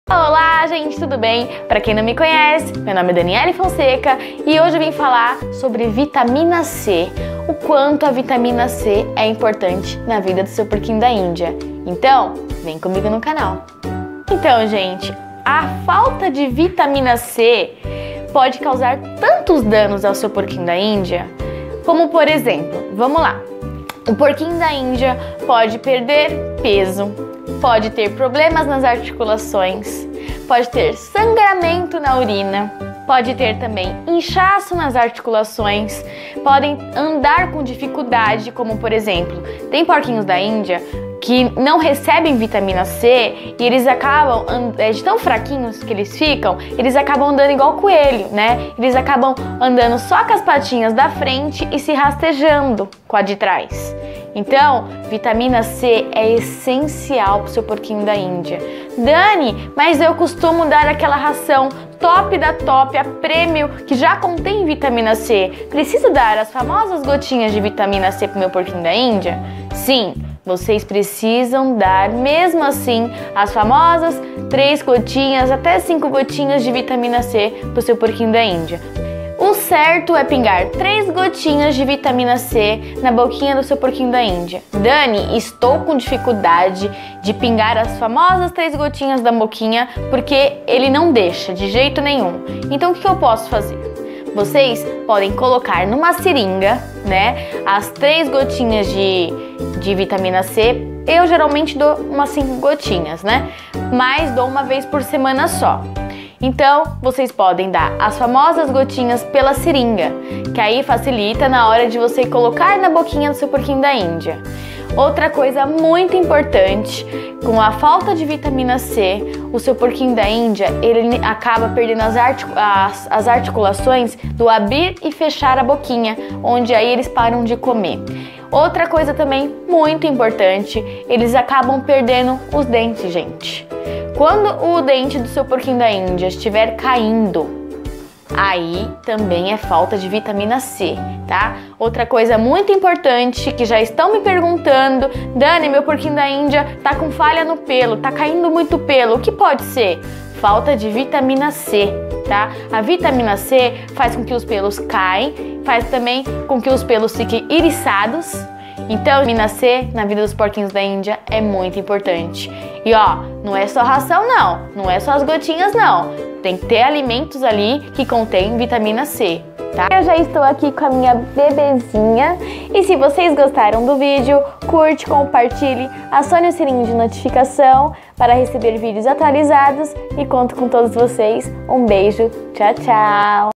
Vitamina C
No vídeo explico a importância da vitamina C, na vida dos Porquinhos Da Índia.
Deixe seu curtir
Siga nosso insta @danielefonseca_manu 😊
Até o próximo vídeo !